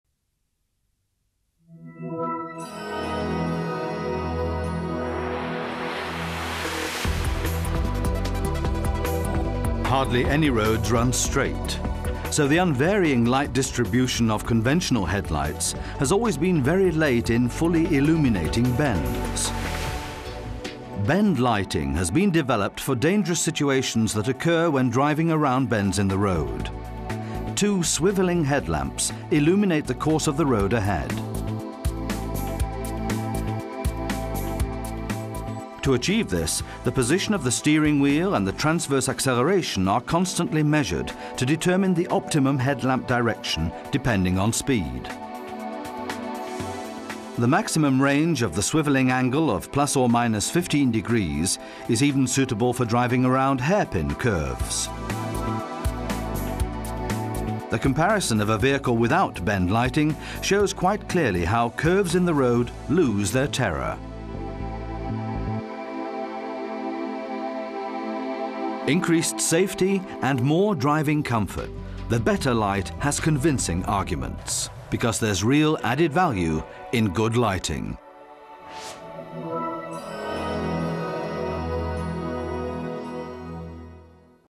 HELLA Dynamic Bend Lighting - Optimal Illumination of Curves
The dynamic bend lighting provides optimal illumination on winding roads due to the horizontal movement of the headlamps. For this purpose both the positioning of the steering wheel and the yaw rate are steadily determined and, depending on the speed, are used for the optimal alignment of the headlamps. Using the bend lighting the visibility is increased significantly and dangerous situations on winding roads can be detected early on.